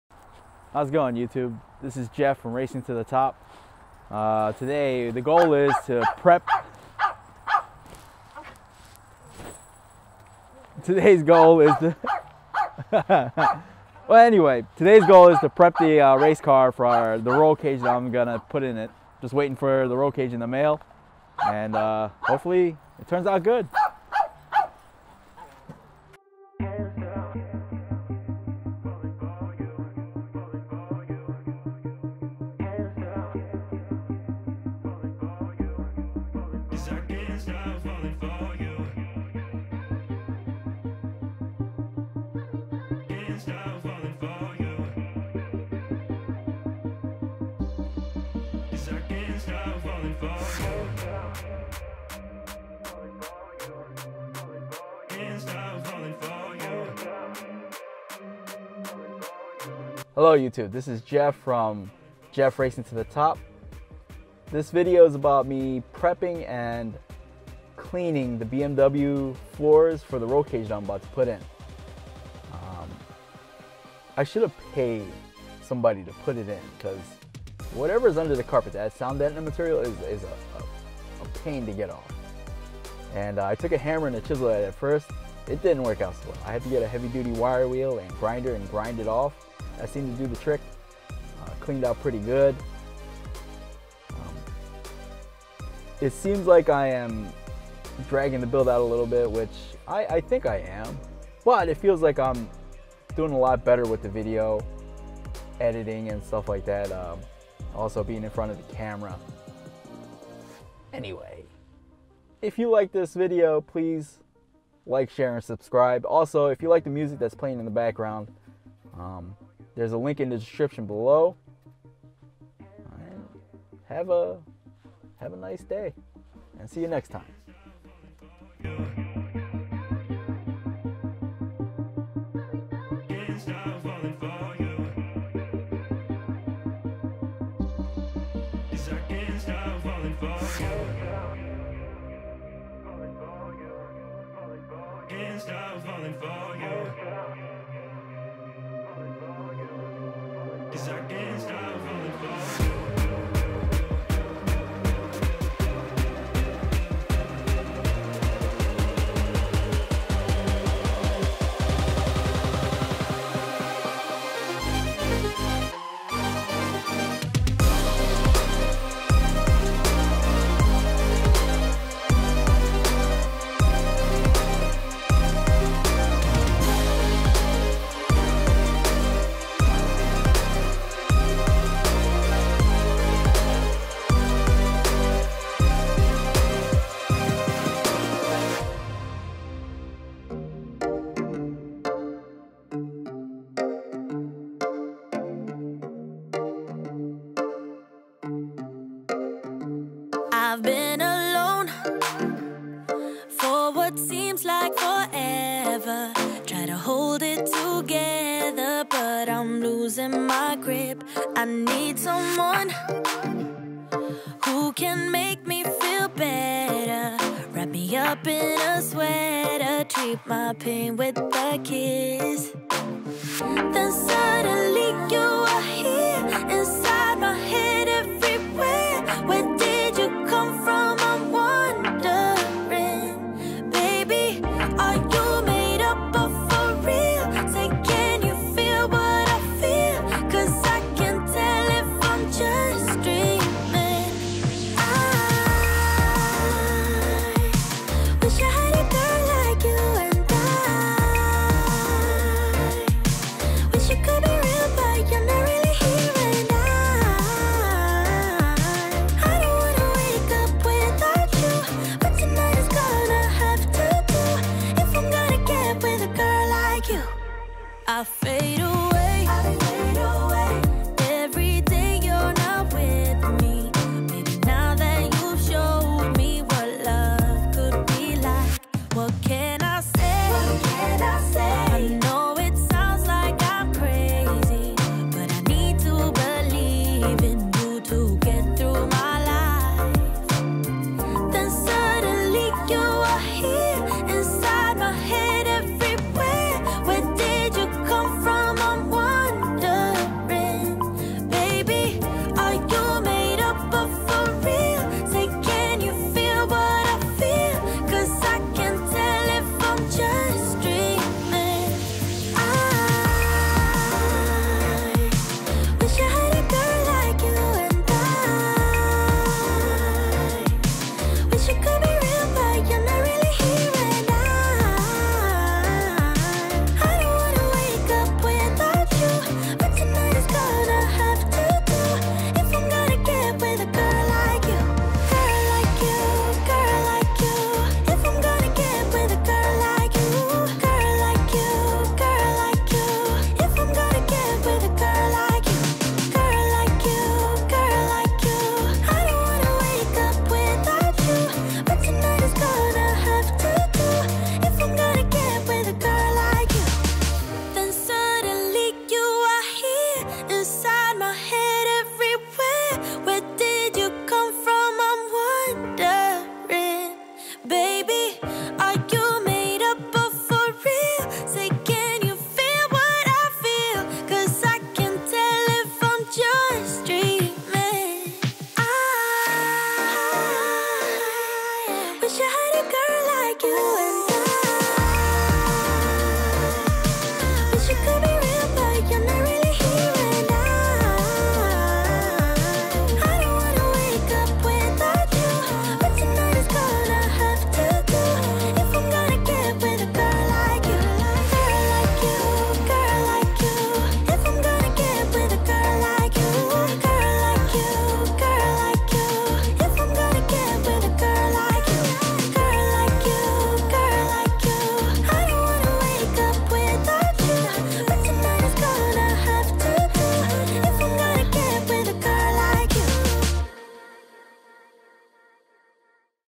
Race car roll cage prep
Getting race car ready for roll cage.